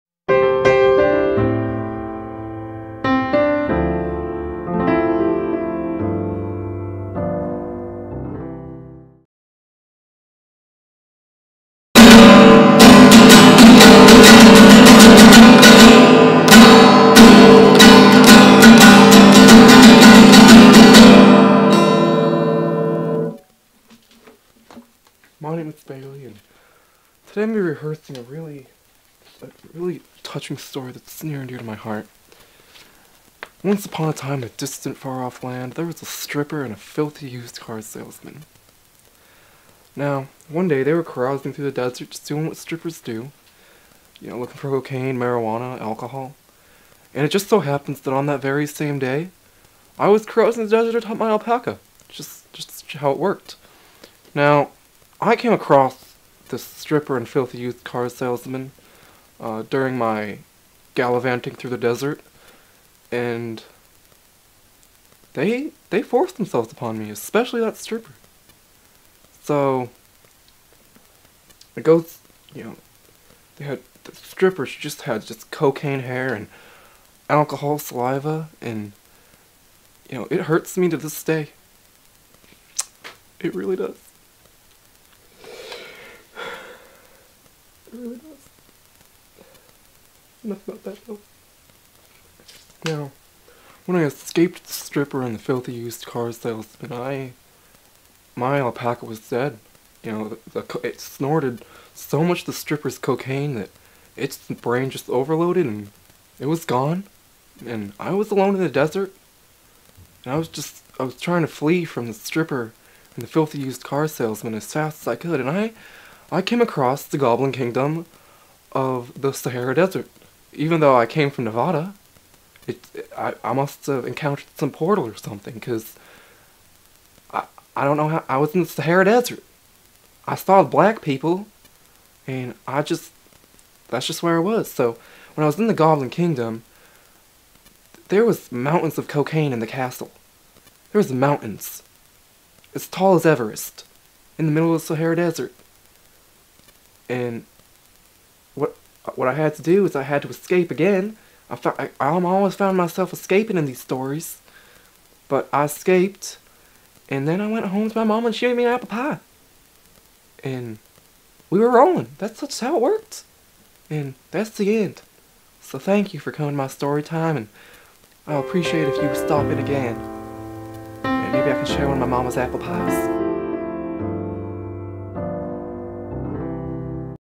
Story Time: Bailey James
Alpacas in the goblin kingdom the sahara. Used sailspersons and strippers. An adventure of adventures. COMPLETELY IMPROVISED.